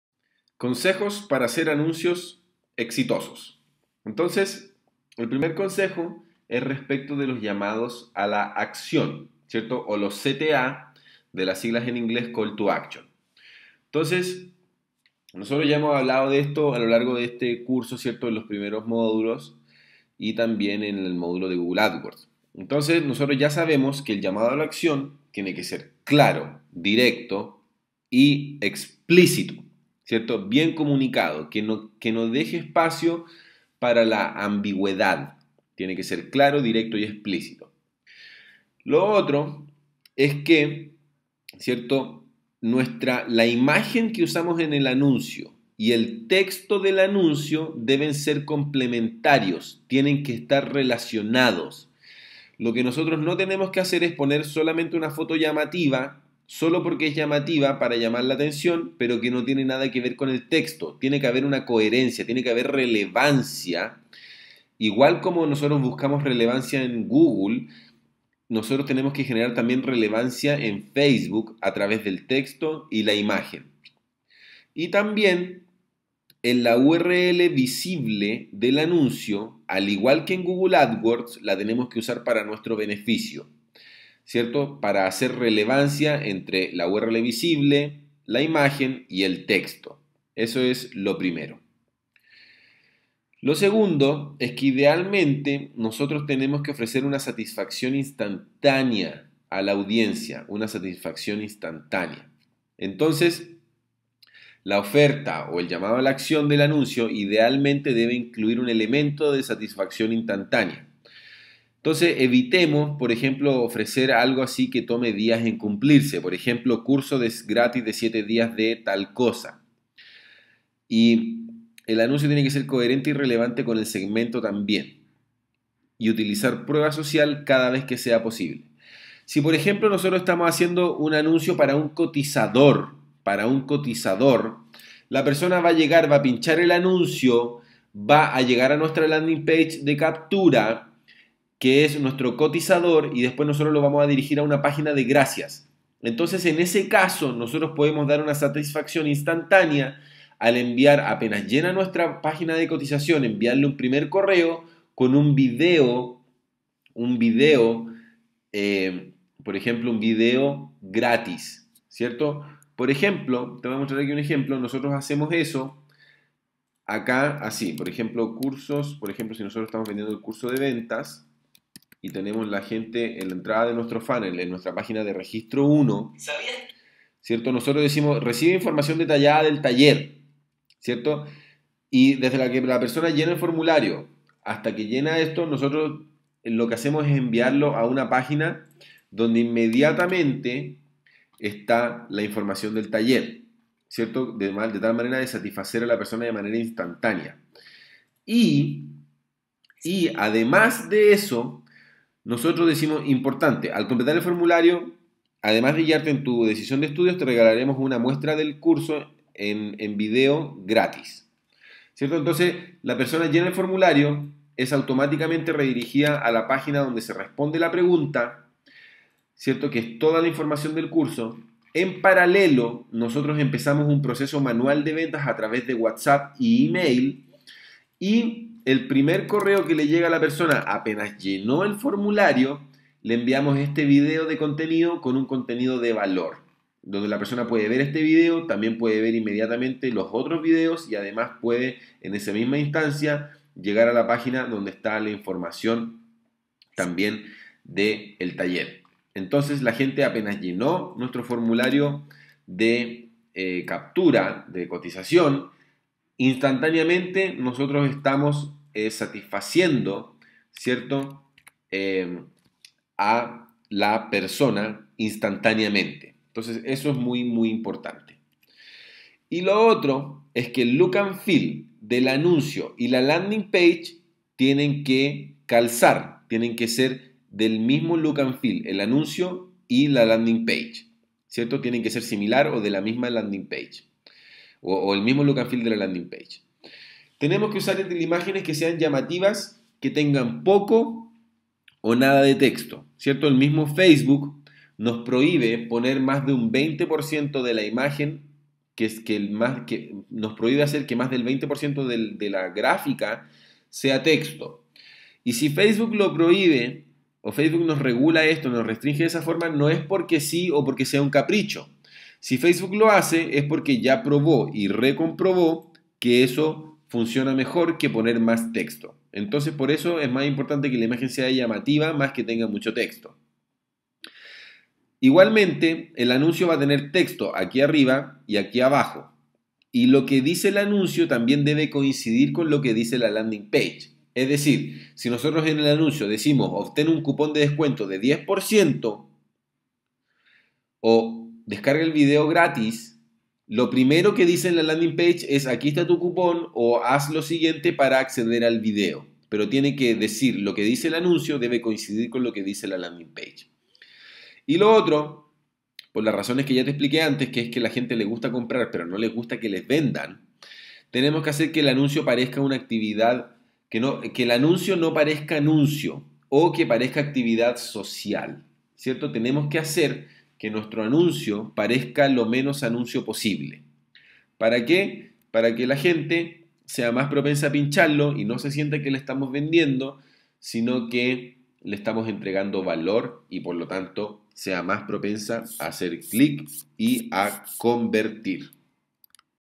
Cómo Hacer Anuncios Exitosos en Facebook Ads
Descubra los mejores consejos para crear anuncios rentables y exitosos en la plataforma de publicidad de Facebook Ads, para mejorar el CTR y Costos por Lead de sus campañas de publicidad.